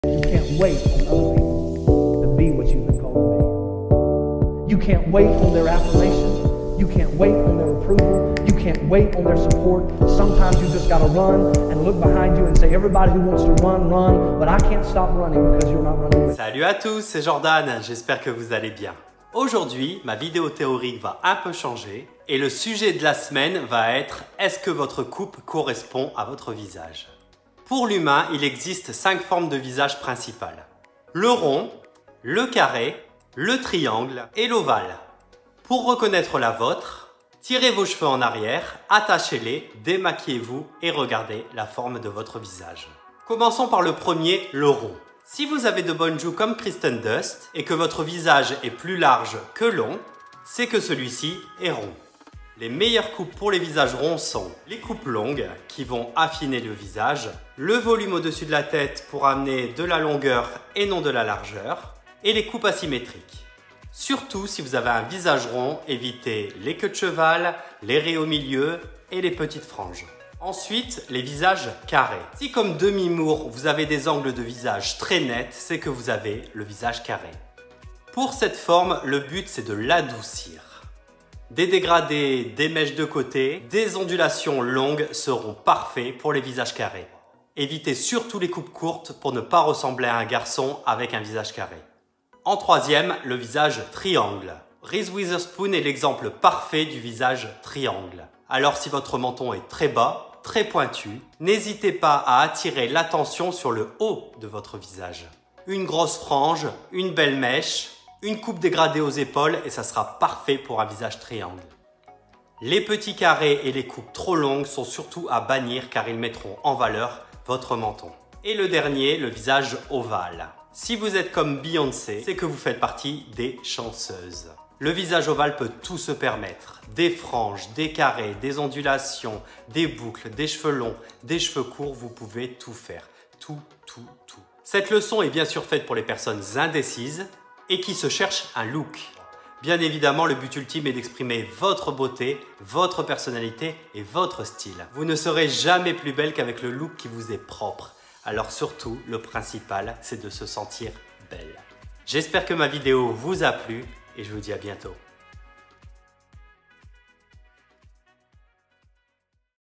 📕 Leçon 26 : La forme du Visage 📕
Dans cette video je t’explique les coupes qui sont adaptés aux formes de visage